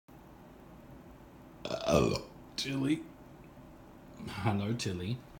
Burrrp! Tillie! (2019-02-19)
A quick video and the last one we have of Chip before he had "The Incident". Unfortunate it wasn't something a little more classy like Chip would have been used to...